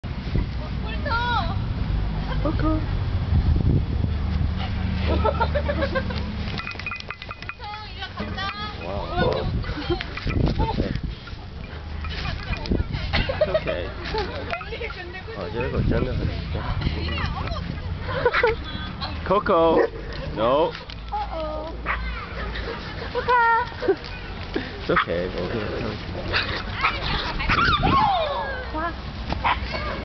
Koko meets Mr. Muggs
My little mongrel dog in Beijing befriends a big English sheep dog.